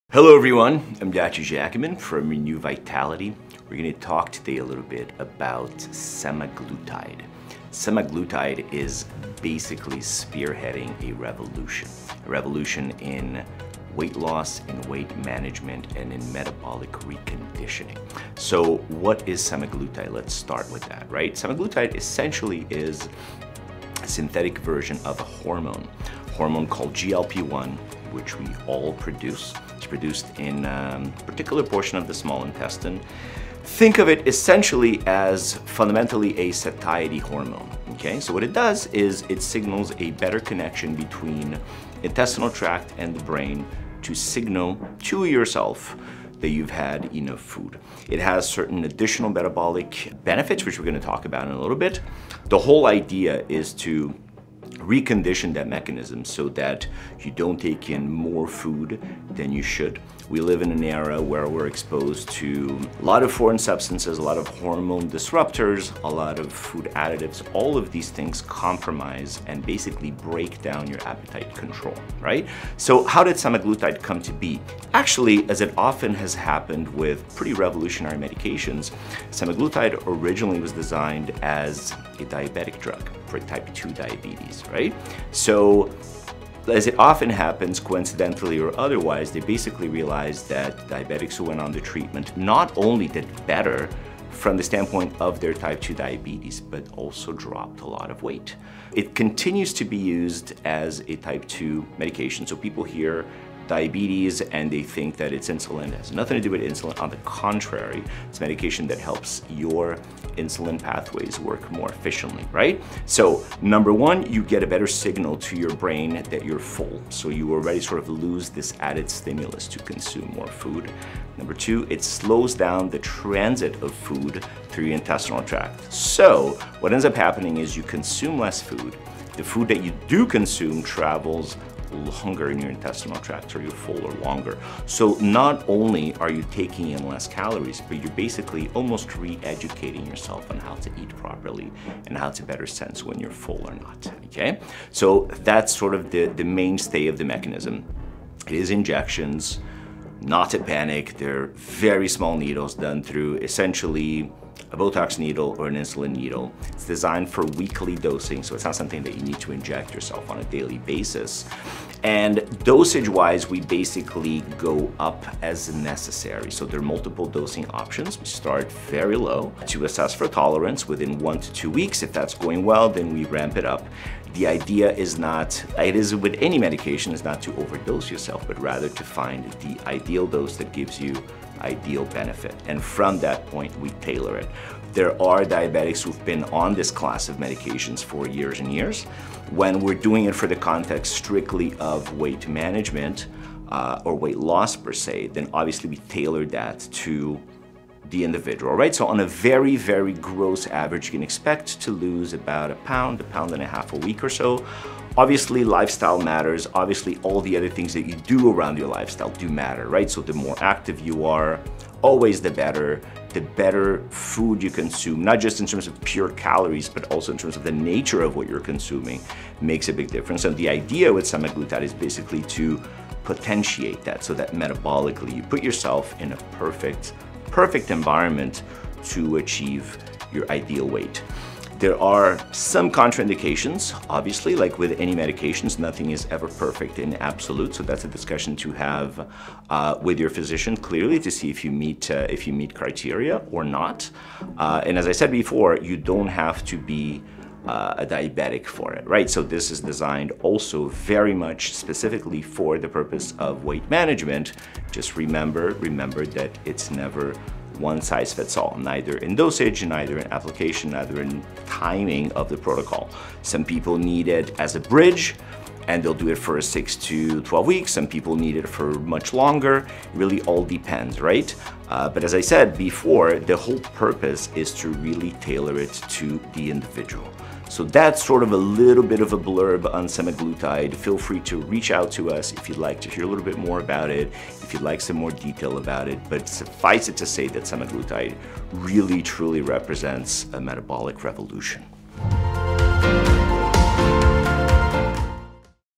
Semaglutide | Revolutionary Weight Loss! | Renew Vitality
Renew Vitality is excited to announce a new revolutionary weight loss medication now available called Semaglutide. Semaglutide is a modified version of a molecule, or protein, that is already present in our bodies, called GLP1. This protein, created in the intestine, promotes a decrease in blood sugars and appetite.

 *Delays stomach emptying which leads to a feeling of fullness and satisfaction with smaller meal sizes

 *Slows intestinal motility

 *Lowers blood sugars, in part by reducing the production of sugar in the liver and stimulates insulin secretion by the pancreas

     *Feel full faster
     *Slower digestion through stomach
     *Better Sleep Patterns
     *Increases Leptin Levels
     *Lowers Blood Sugars
     *Diminishes Hunger and Cravings
     *Helps break down Insulin Resistance
     *Lowers A1c Levels
     *Suppresses Appetite
     *Creates Consistency in Weight Loss